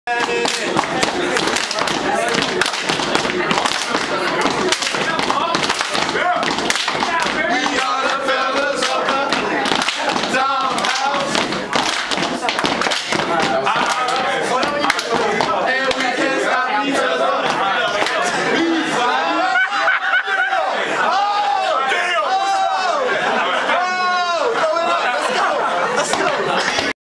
Teaching Zahm to step
First Class tries to teach Zahm How to step. This is what happens